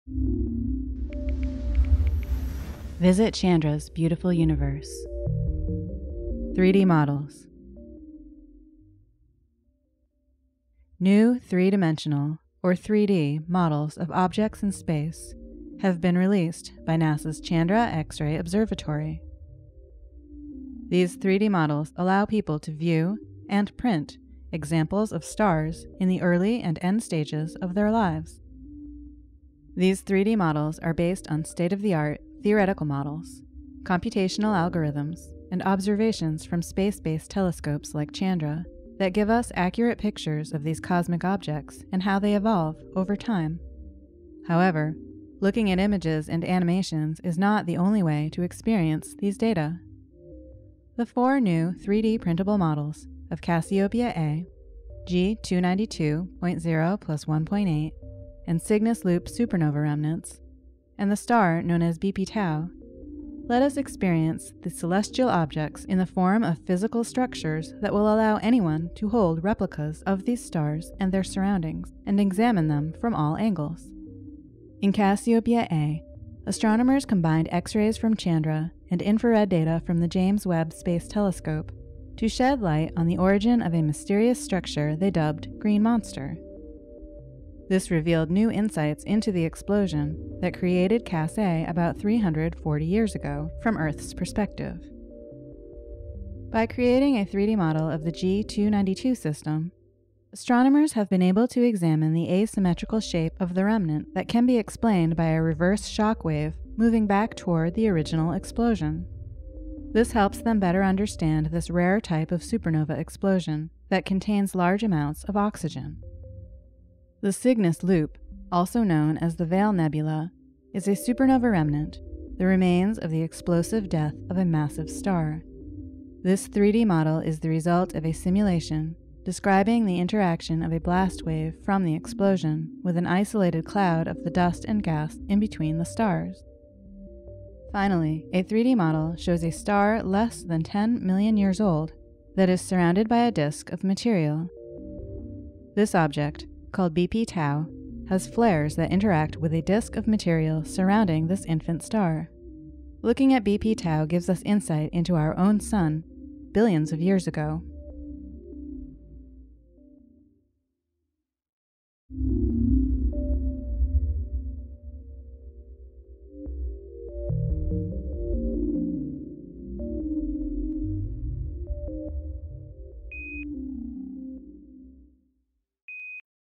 Tour: NASA's Chandra Releases New 3D Models of Cosmic Objects
New three-dimensional, or 3D, models of objects in space have been released by NASA’s Chandra X-ray Observatory. These 3D models allow people to view — and print — examples of stars in the early and end stages of their lives.

These 3D models are based on state-of-the-art theoretical models, computational algorithms, and observations from space-based telescopes like Chandra that give us accurate pictures of these cosmic objects and how they evolve over time.

However, looking at images and animations is not the only way to experience these data. The four new 3D printable models of Cassiopeia A, G292.0+1.8, and Cygnus Loop supernova remnants and the star known as BP Tau let us experience the celestial objects in the form of physical structures that will allow anyone to hold replicas of these stars and their surroundings and examine them from all angles.

In Cassiopeia A, astronomers combined X-rays from Chandra and infrared data from the James Webb Space Telescope to shed light on the origin of a mysterious structure they dubbed Green Monster. This revealed new insights into the explosion that created Cas A about 340 years ago, from Earth’s perspective.

By creating a 3D model of the G292 system, astronomers have been able to examine the asymmetrical shape of the remnant that can be explained by a reverse shock wave moving back toward the original explosion. This helps them better understand this rare type of supernova explosion that contains large amounts of oxygen.

The Cygnus Loop, also known as the Veil Nebula, is a supernova remnant, the remains of the explosive death of a massive star. This 3D model is the result of a simulation describing the interaction of a blast wave from the explosion with an isolated cloud of the dust and gas in between the stars.

Finally, a 3D model shows a star less than 10 million years old that is surrounded by a disk of material. This object, called BP Tau, has flares that interact with a disk of material surrounding this infant star. Looking at BP Tau gives us insight into our own Sun billions of years ago.